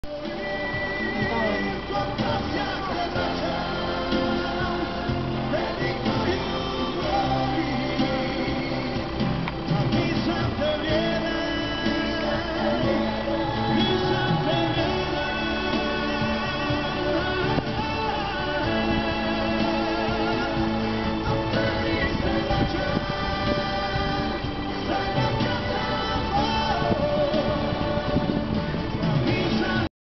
Paragliding Macedonia Meckin Kamen Ilinden
Uspomena na predivno popodne na Meckinom Kamenu, Krusevo, Macedonia.